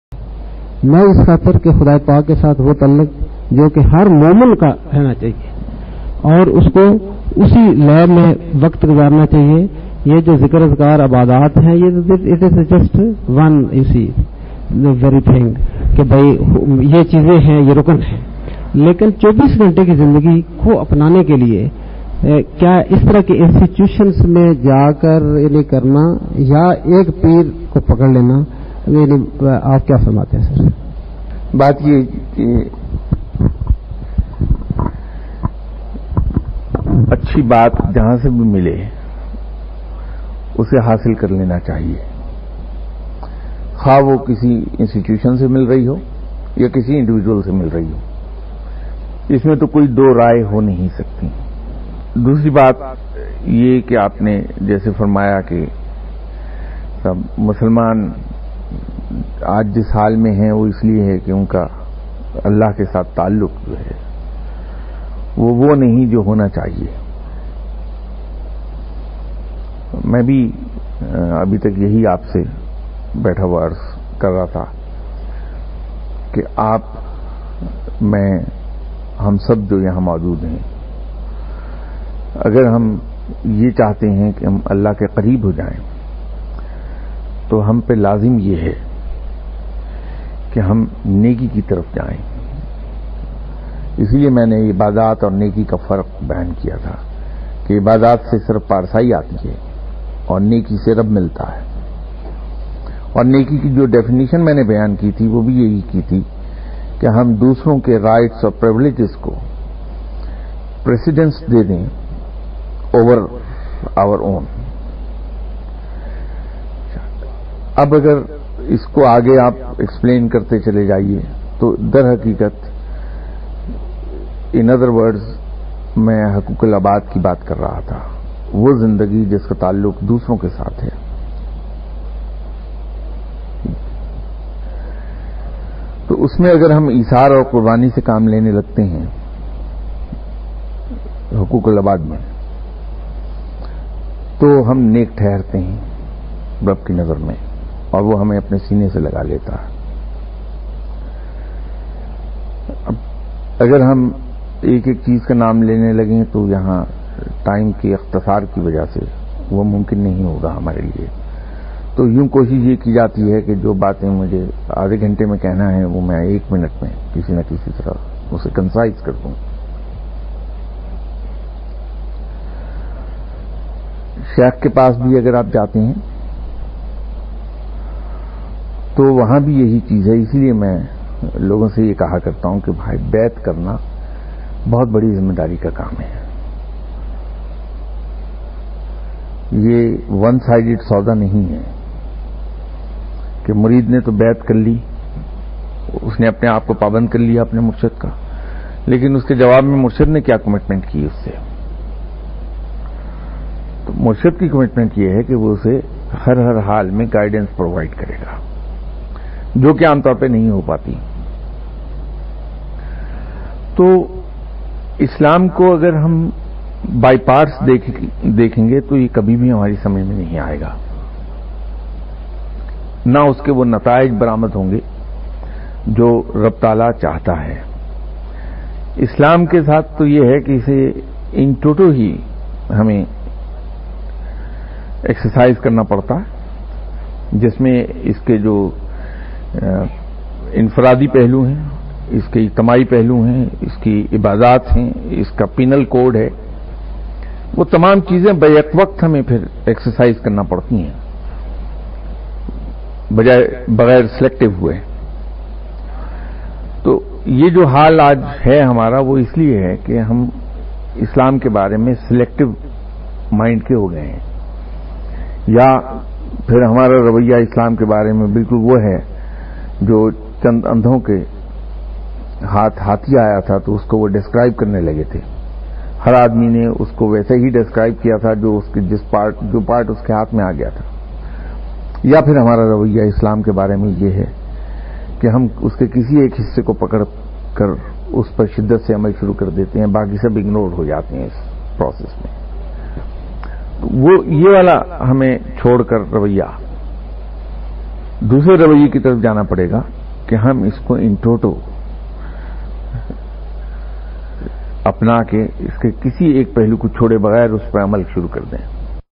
Lecture 5 - Part 4 of 4 - Be kind to others - 09-07-2006 - Lectures by Mr. Sarfraz A. Shah
Lecture 5 - Topic: Be kind to others and Allah will be kind to you. Do good to others even in response to bad.
Lectures in Urdu by Mr. Sarfraz A. Shah